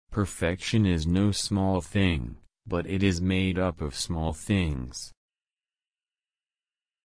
Michelangelo | Inspirational quote #1
Michelangelo di Lodovico Buonarroti Simoni (1475 – 1564), known as Michelangelo, was an Italian sculptor, painter, architect, and poet of the High Renaissance. Born in the Republic of Florence, his work was inspired by models from classical antiquity and had a lasting influence on Western art. Michelangelo's creative abilities and mastery in a range of artistic arenas define him as an archetypal Renaissance man, along with his rival and elder contemporary, Leonardo da Vinci.[2] Given the sheer volume of surviving correspondence, sketches, and reminiscences, Michelangelo is one of the best-documented artists of the 16th century. He was lauded by contemporary biographers as the most accomplished artist of his era.